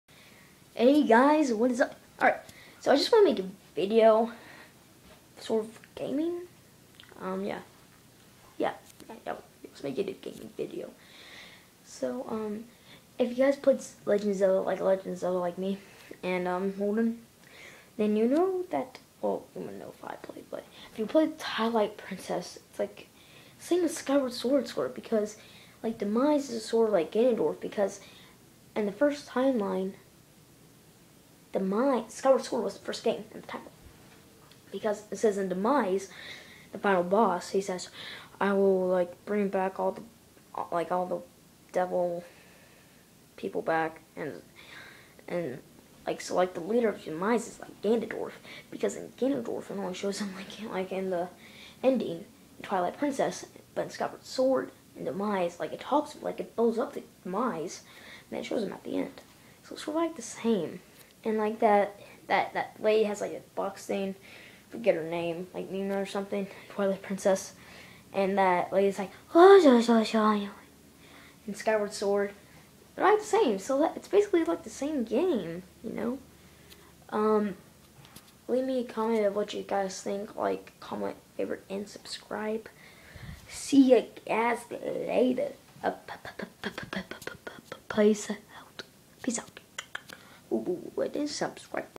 Gaming video
Taking about legend of Zelda